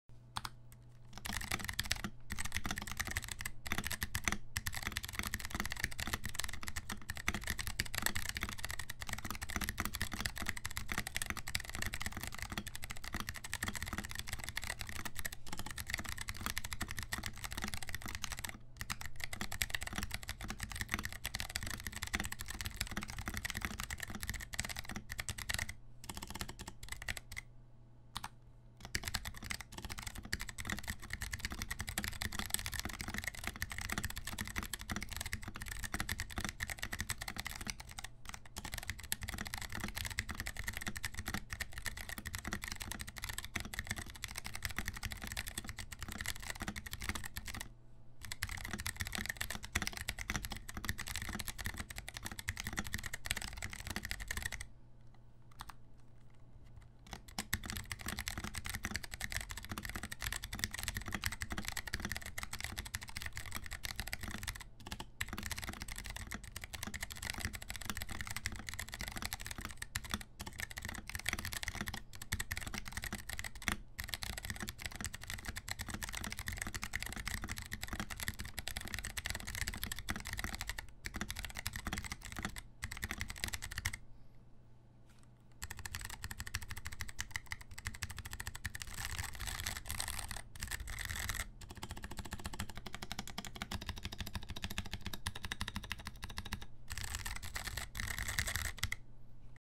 Kailh BOX Mute Jade sound test (NK65 Entry)
My webcam doesn't focus on objects from that far away apparently.

The switches are actually pretty nice. Sound is slightly disappointing (although I'm not exactly sure what I was expecting from a silent clicky switch) and it's not cool how these don't really feel like the switch they're supposed to be a variant of at all, but it's pretty nice to have a muted sound coming from a switch that mostly feels like a clicky switch.